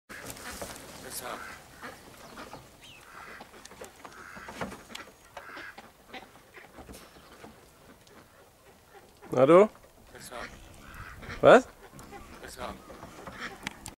wichser
Unser Kolkrabe kann sich nicht benehmen !!! Für die "Daumen runter Drücker" ...WIR haben ihm das nicht beigebracht ! Er kam so zu uns in die Station!!!